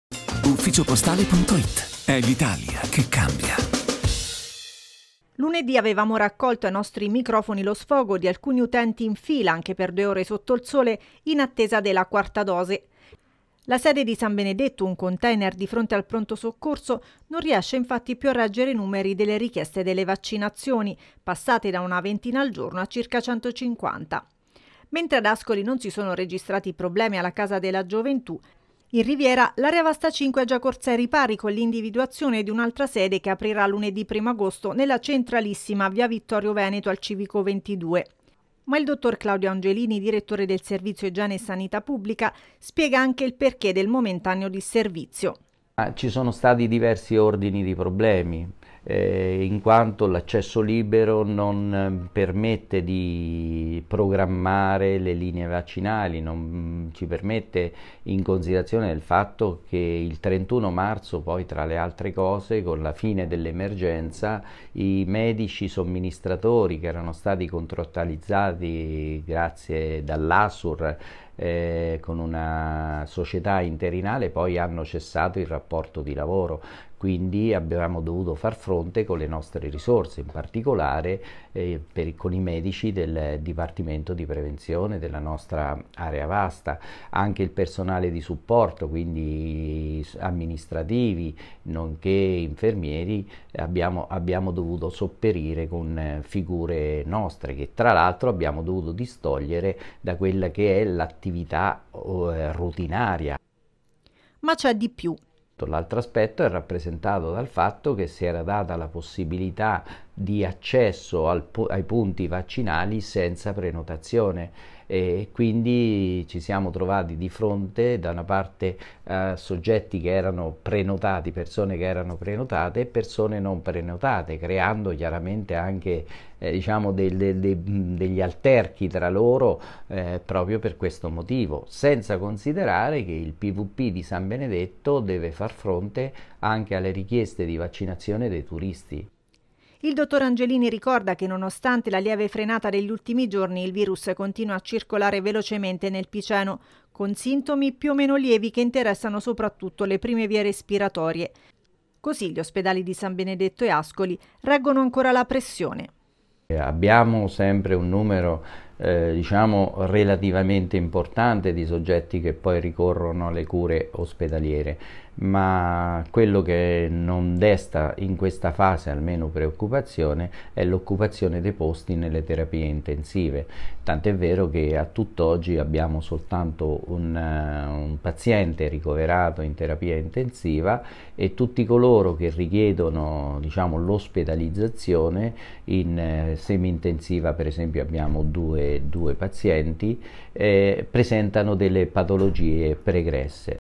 Code per i vaccini, l'Area Vasta 5: "Pochi medici e turisti senza prenotazione"
Anziani in fila con il caldo record